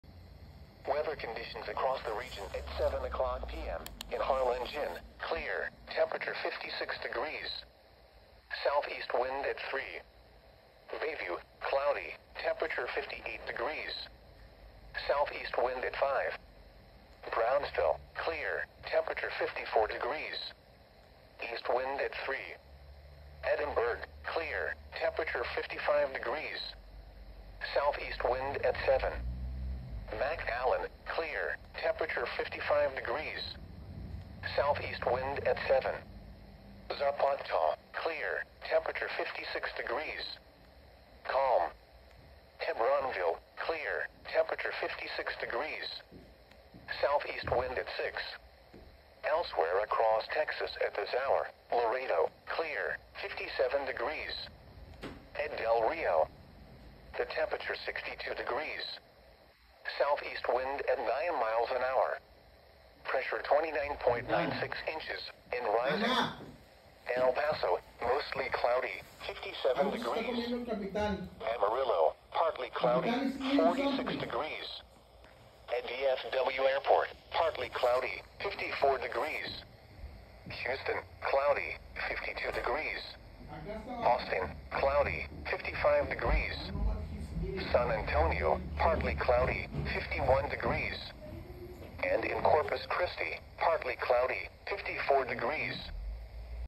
NOAA weather radio weather conditions across the region at 7 PM ￼
Recorded just now enjoy ￼